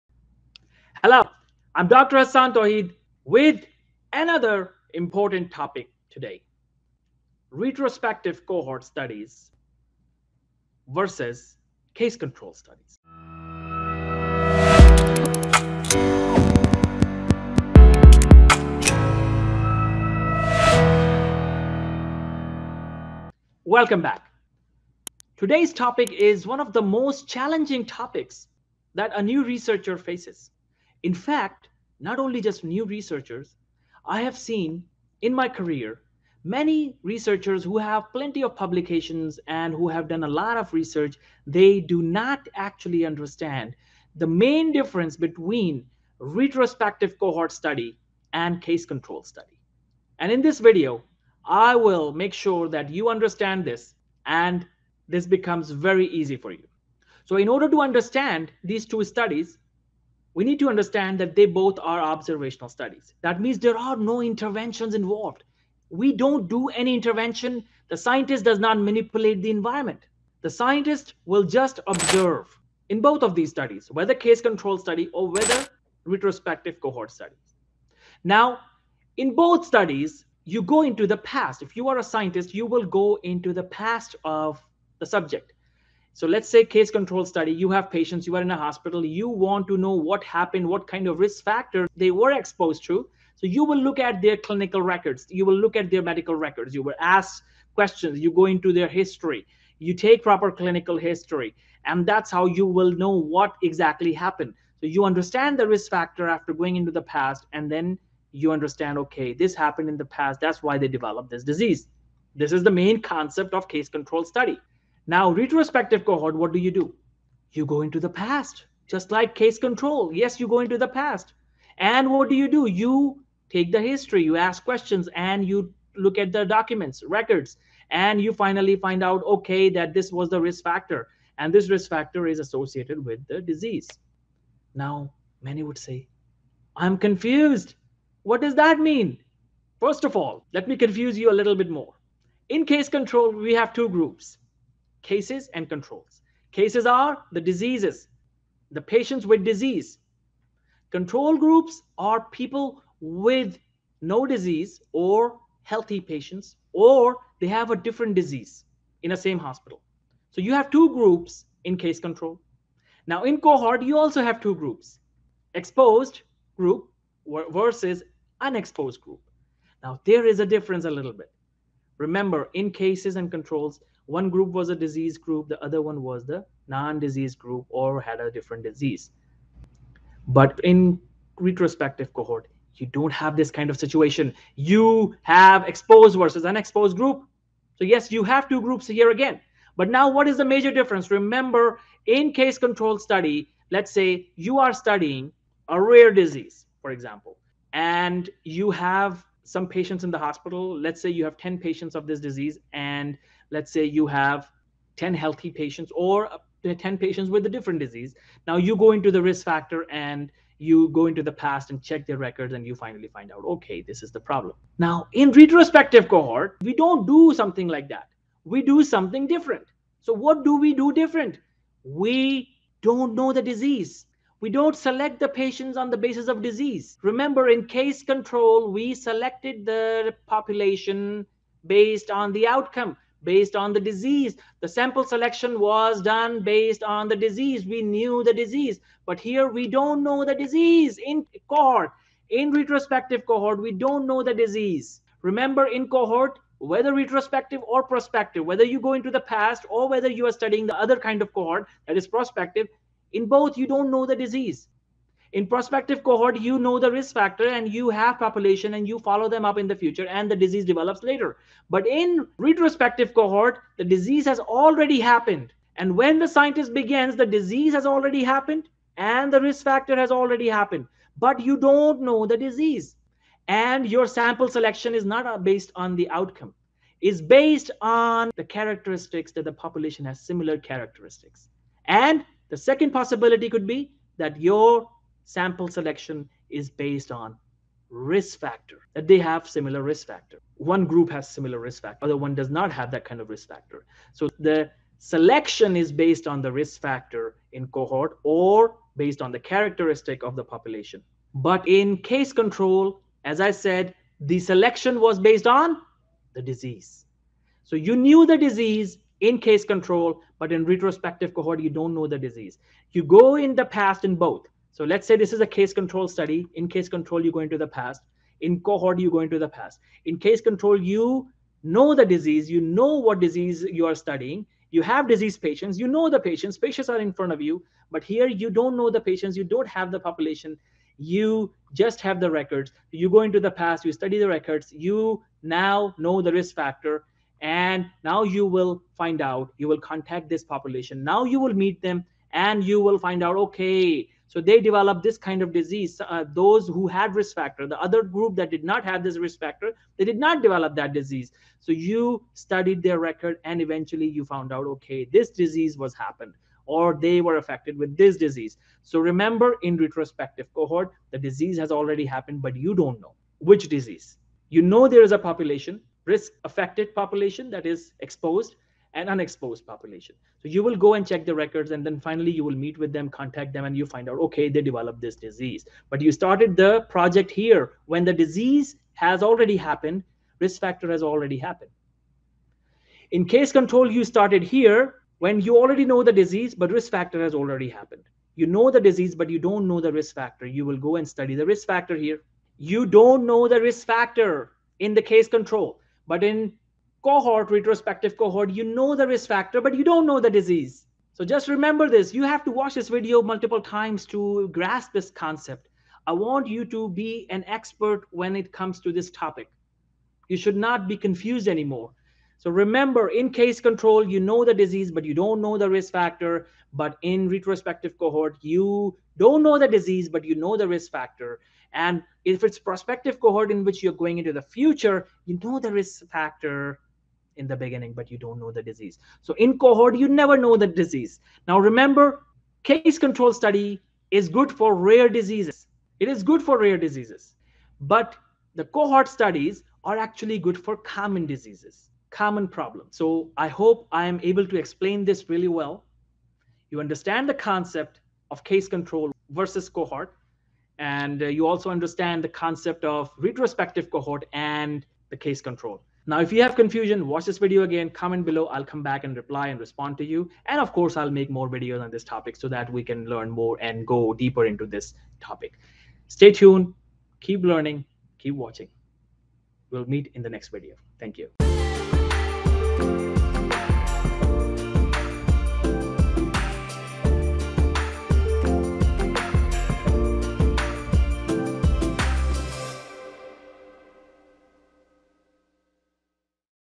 Retrospective Cohort vs Case Control Study?
If you're having trouble to differentiate the RESTROSPECTIVE COHORT STUDY from the CASE CONTROL STUDY, you are in the right place.

This is one of the most challenging topics for new researchers or even for professionals with years of experience. that's why in this video, Dr. Hassaan Tohid will make this topic easy to understand, so you can do a better development in your work area.

If you want us to talk about a specific topic, or have any question, comment in the box bellow!